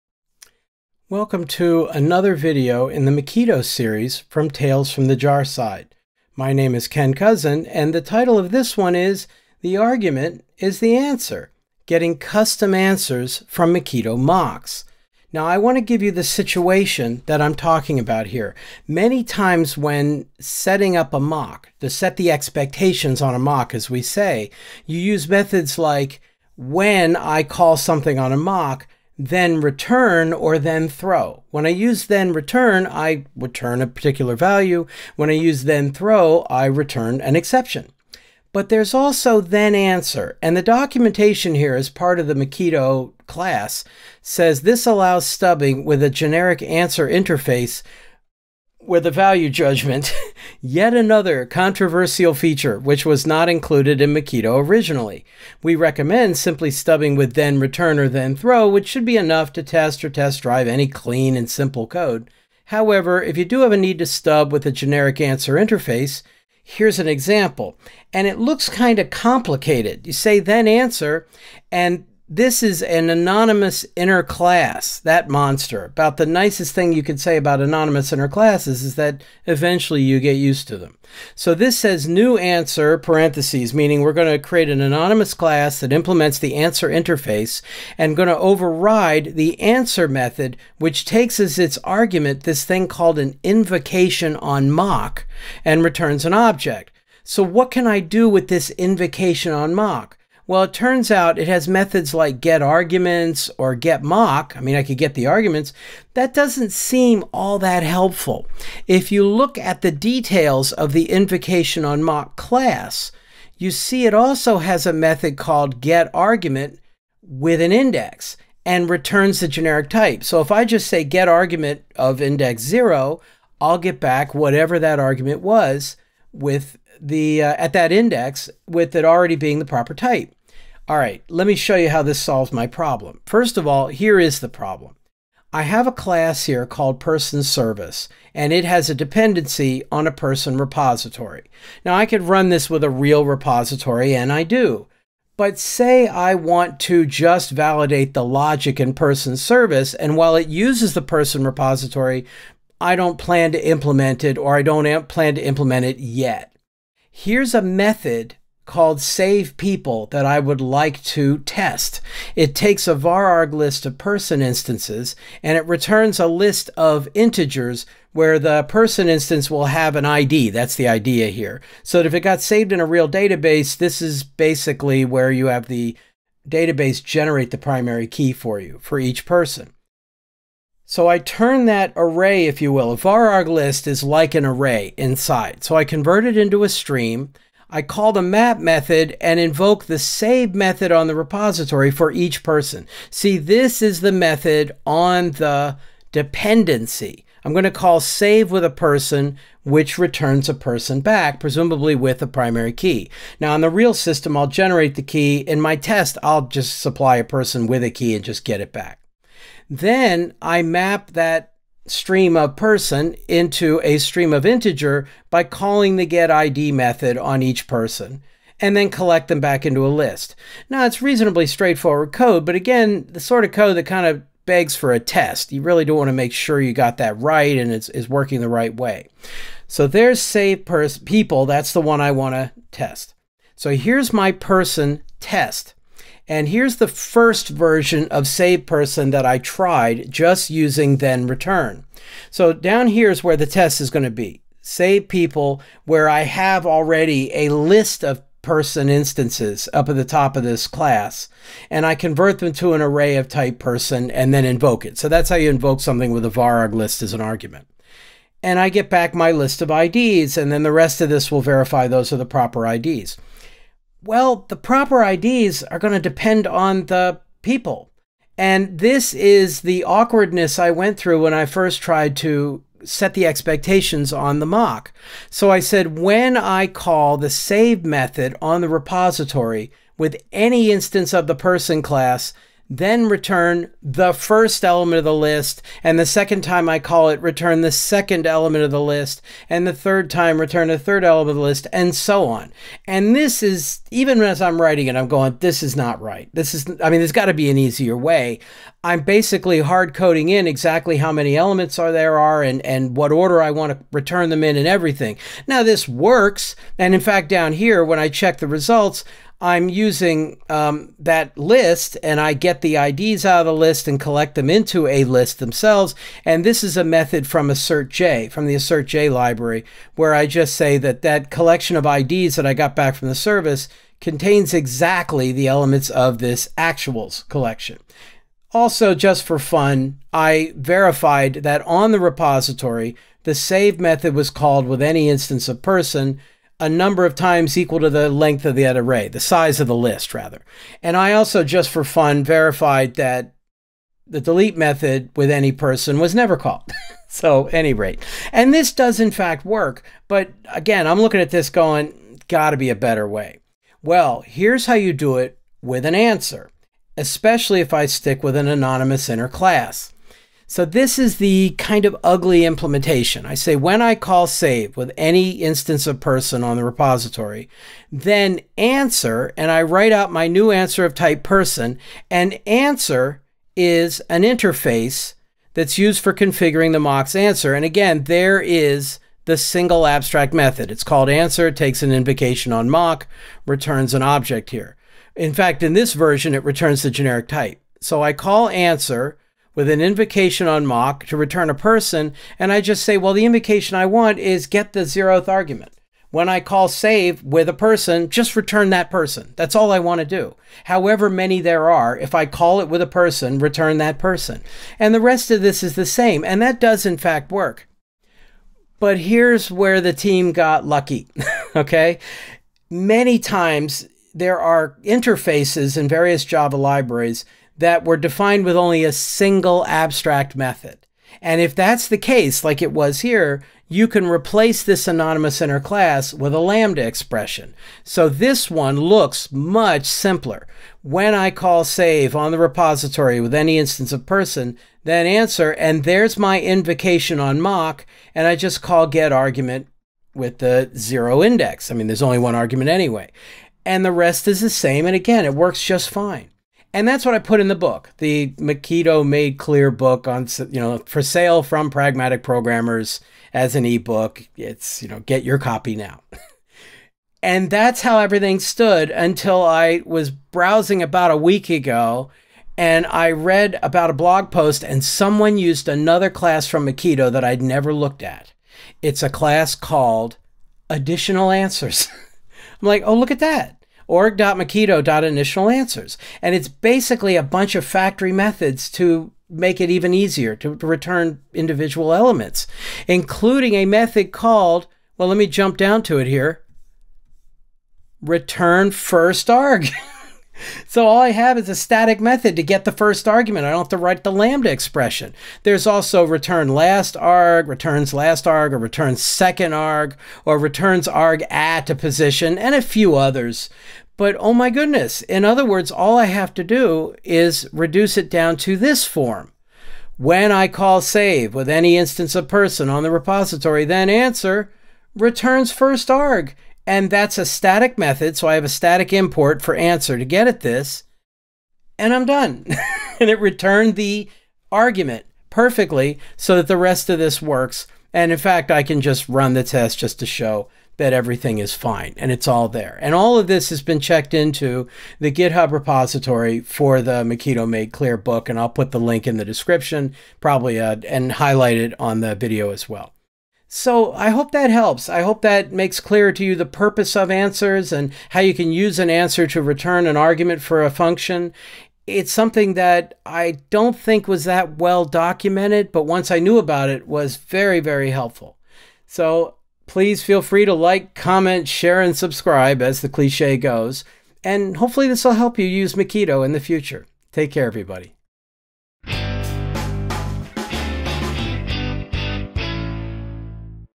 The (Mockito) Answer Is The Argument
Set expectations on a Mockito mock using the thenAnswer() method. Implements the Answer interface three ways: once using an anonymous inner class, once using a lambda expression, and finally using a method from the AdditionalAnswers class.

00:00 - First Marker
02:15 - PersonService and Test
05:55 - Anonymous Inner Class
06:48 - Implement Answer with Lambda
07:50 - AdditionalAnswers.returnsFirstArg()